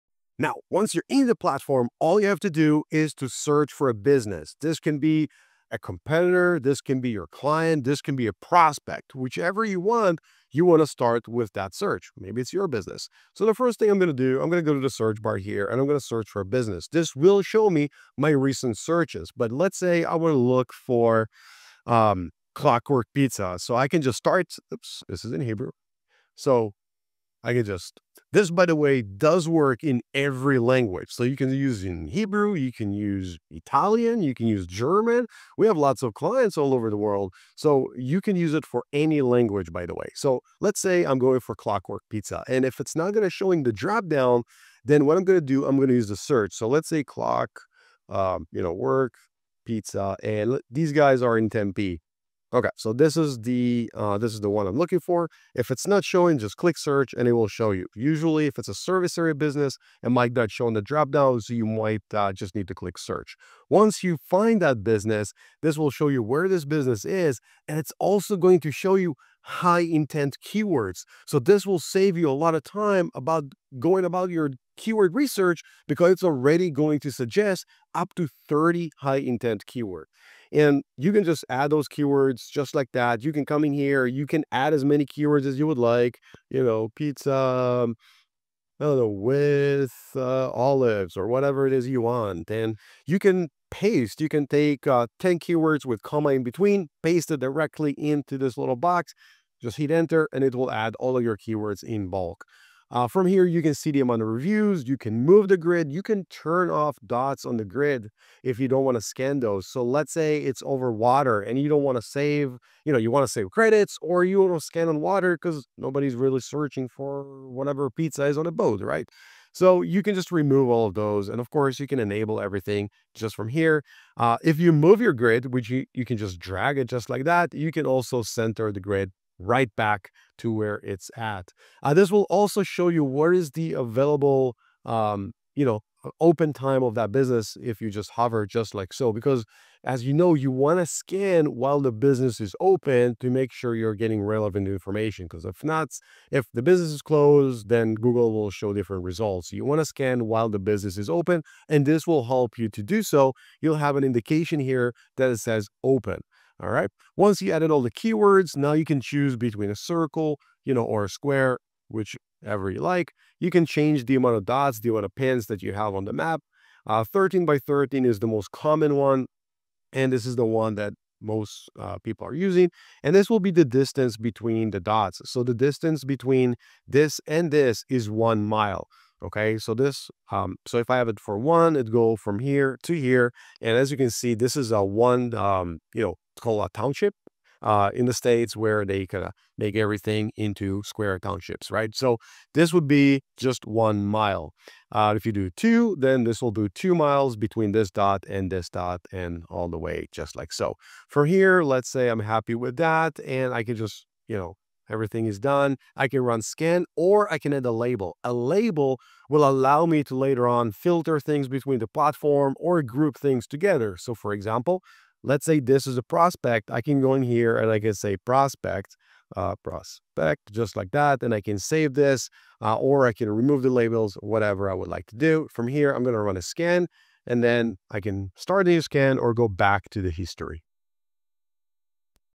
2. How to Search, Add Keywords & Run Your First Scan
🔥 Ready to start scanning? In this video, Eldar walks you through exactly how to search for a business and run your very first scan inside Local Dominator. Whether it’s your client, a competitor, or a prospect—you’ll see how quick and easy it is to get started.

👨‍💻 What you’ll learn:
 • How to search for any business (including SABs)
 • What to do if the business doesn’t show in the dropdown
 • Using AI-powered keyword suggestions
 • Adding keywords manually or in bulk
 • Customizing your scan grid (square vs. circle, pin distance, disabling water areas)
 • Checking business open hours for accurate scan timing
 • Labeling scans for better organization

💡 Pro Tip: Always scan while the business is open to get the most accurate local ranking data from Google!

📍 Watch this before running your first scan—it’s a must for new users.